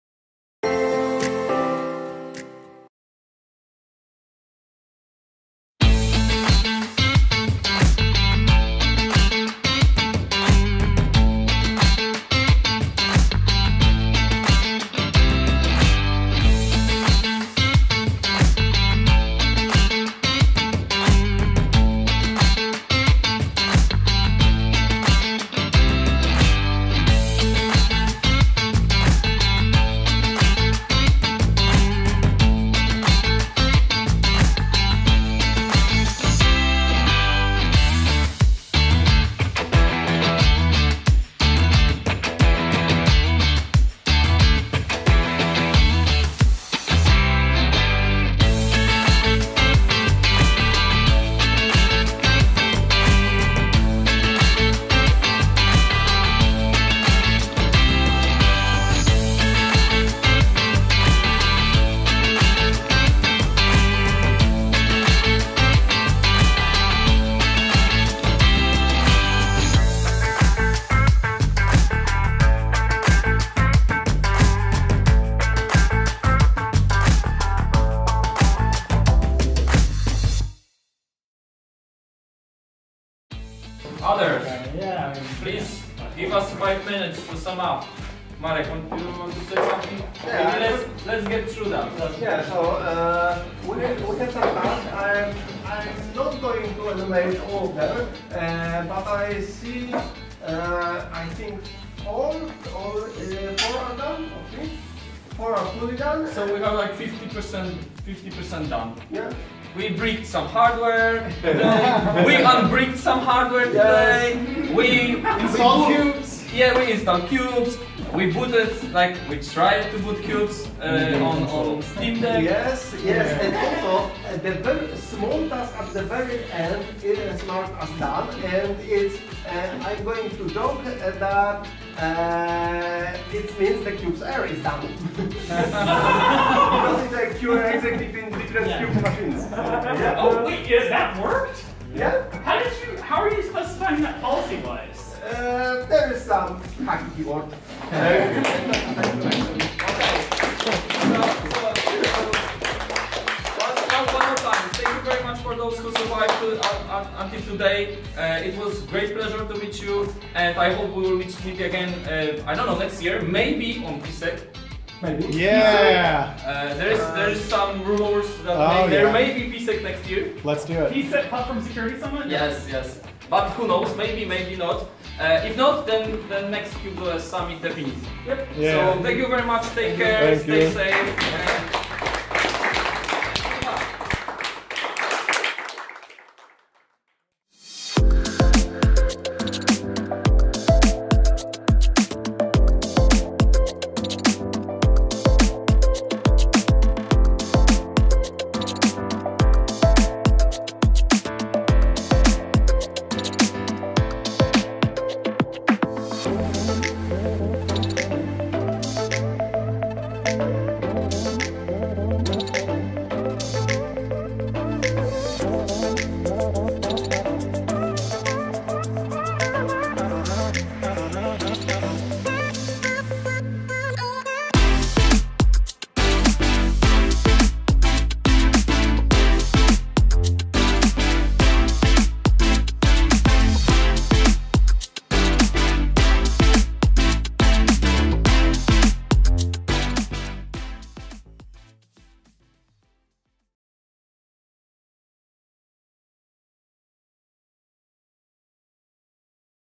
Qubes OS summit 2022: Day 3 - Hackathon
Qubes OS summit 2022 is the event co-hosted with 3mdeb, where the Qubes OS experts will cover various topics related to the design, security, and usability of our favorite operating system.

Qubes OS summit 2022 will be held 9th to 11th of September.

Otherwise, you can visit the 3mdeb event's website to read more about your contribution to Open Source Community.